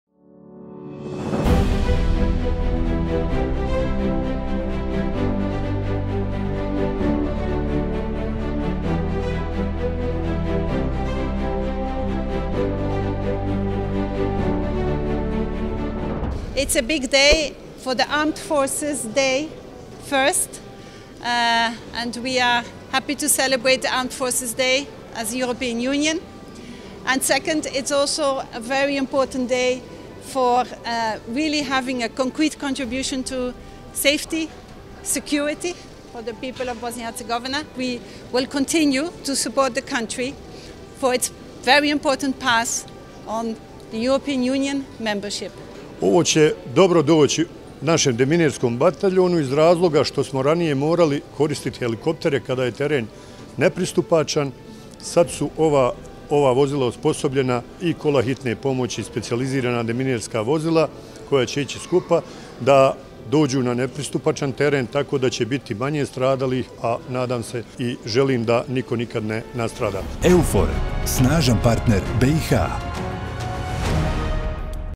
Handover ceremony of demining vehicles and equipment in Camp Rajlovac
COM EUFOR Major General Helmut Habermayer recently attended the AFBiH handover ceremony of demining vehicles and equipment in Camp Rajlovac, Sarajevo. The equipment included specialized demining vehicles and ambulances which was provided by a European Peace Facility project. EUFOR is proud to be a technical partner in this project and looks forward to continued EU / BiH co-operation.